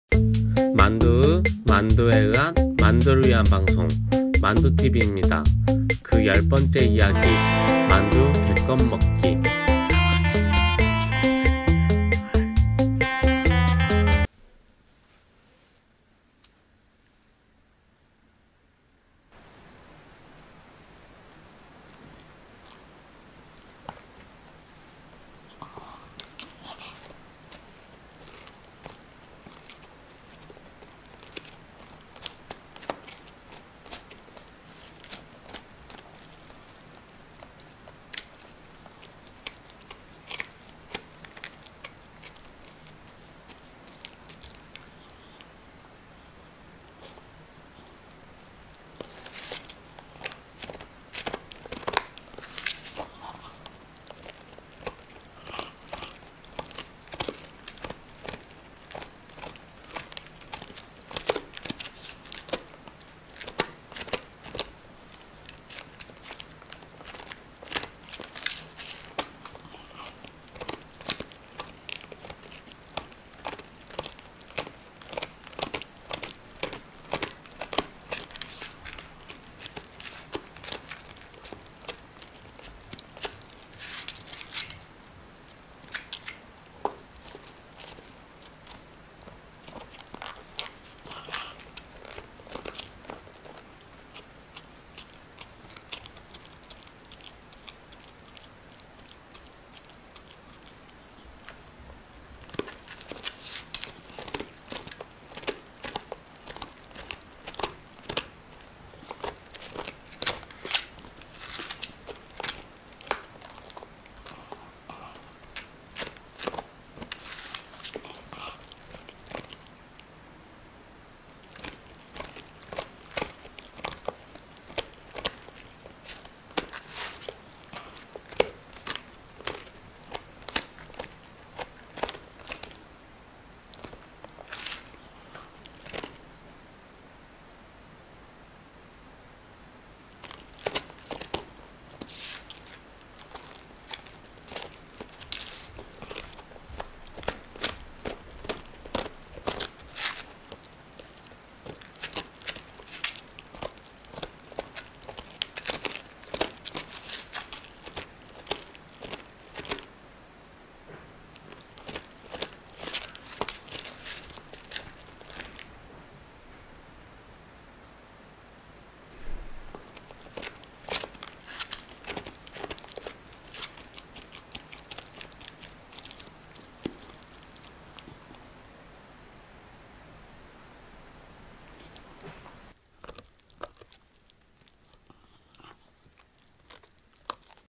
강아지 개껌 먹방 ASMR [슈나우저 Schnauzer 만두tv]
만두 입니다~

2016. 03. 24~
미니어쳐 슈나우져 입니다!!

만두티비 열번째 이야기!!
강아지 asmr, 만두 개껌 먹방